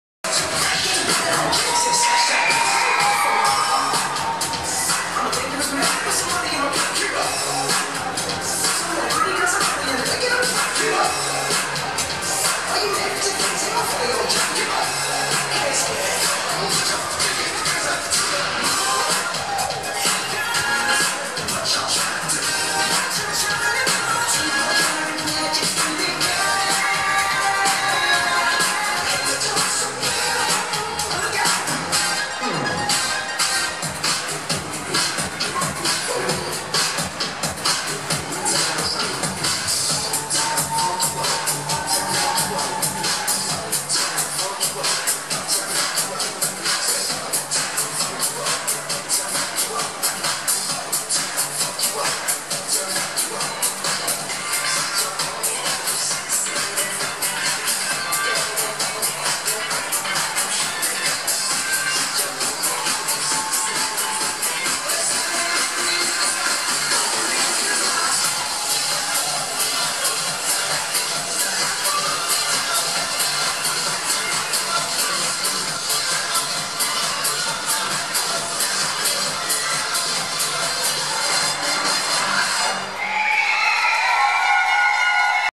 Slovak Championship, Spišská Nová Ves 2017 - SVK Kysucké Nové Mesto, DREAMS
AMAS XV. Slovak Majorette-sport Championship -  SPIŠSKÁ NOVÁ VES/SK, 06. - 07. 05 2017
Mini pom-pom cadet - 4th place (108.40)

SLOVAKIA Kysucké Nové Mesto - DREAMS

Member of AMAS Slovakia